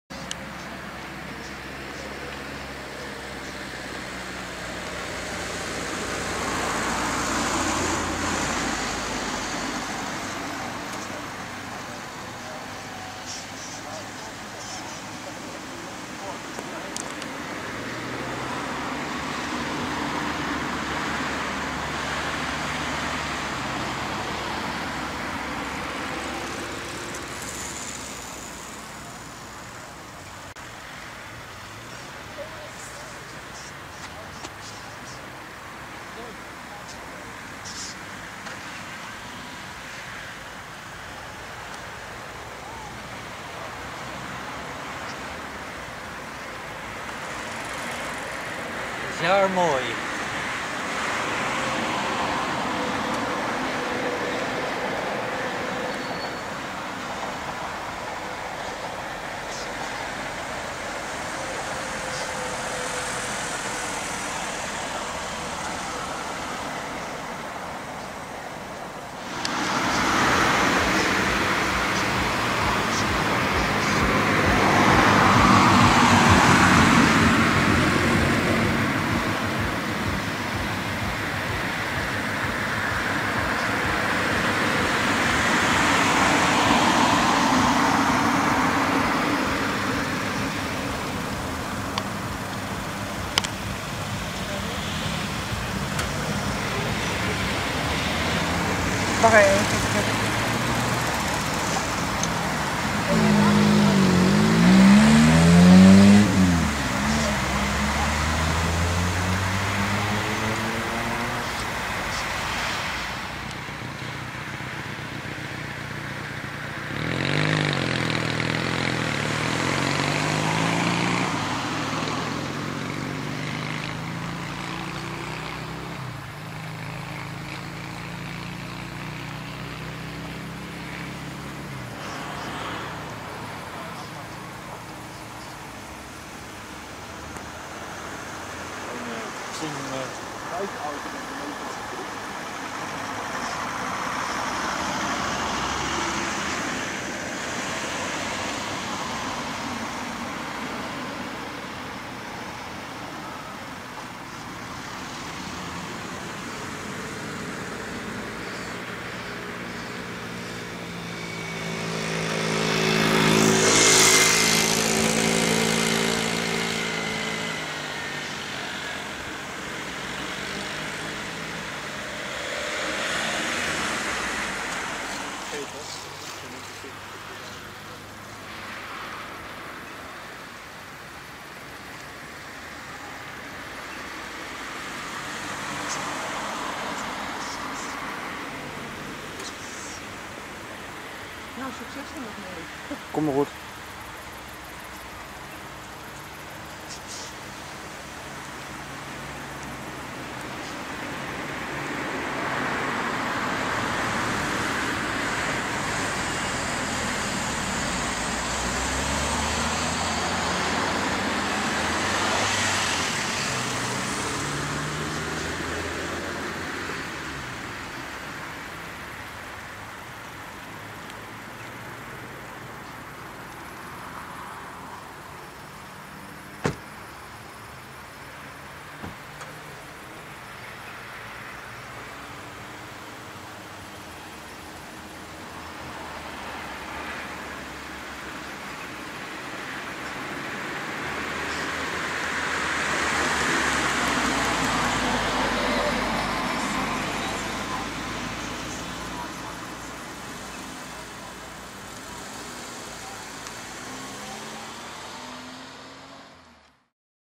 zwenkende zwermen spreeuwen dansen in de lucht 31-08-2014
birds dancing in the sky after sunset :-)